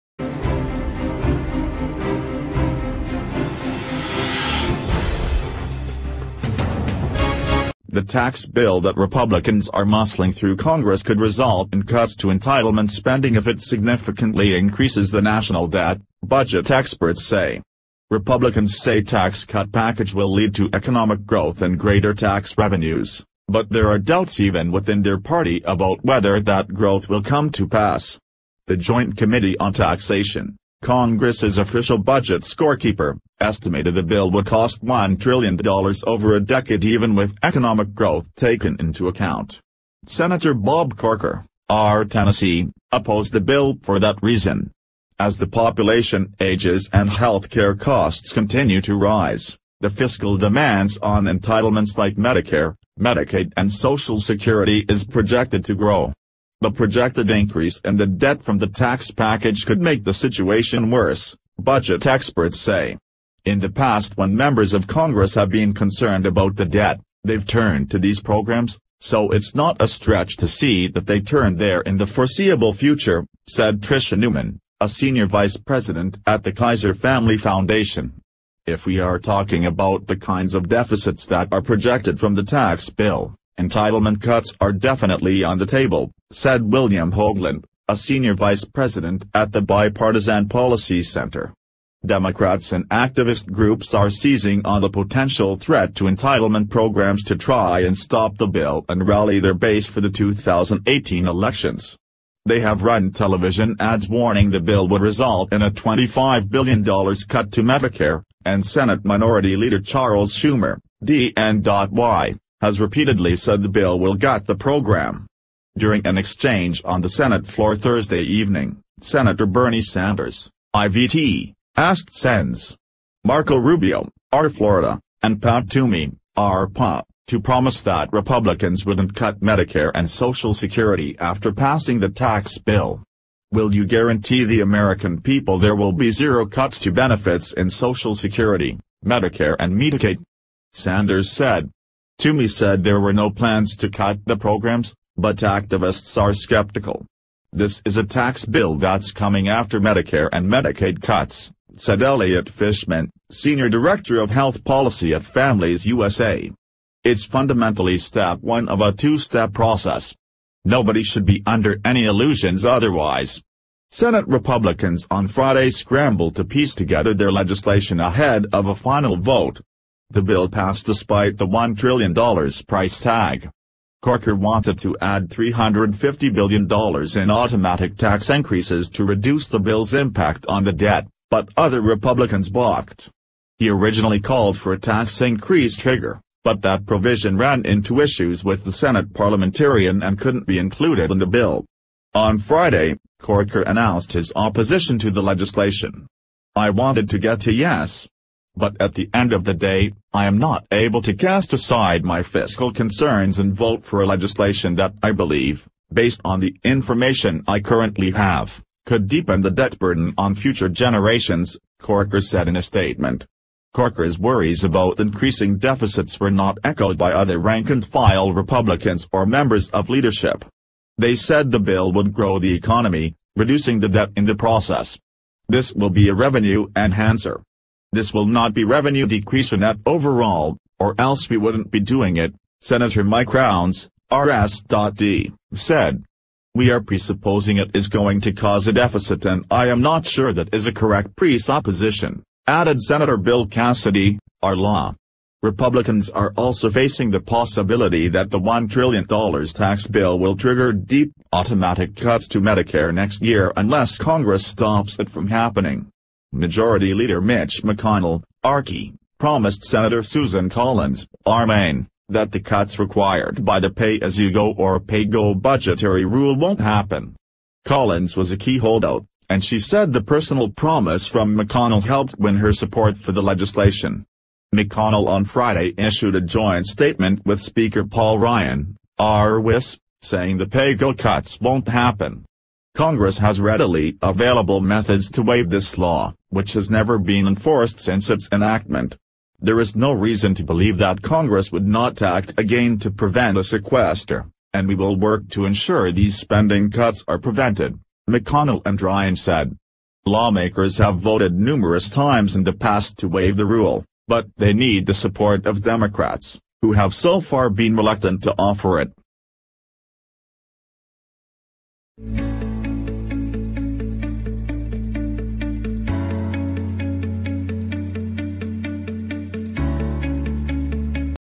Tax bill could fuel push for Medicare, Social Security cuts
The tax bill that Republicans are muscling through Congress could result in cuts to entitlement spending if it significantly increases the national debt, budget experts say.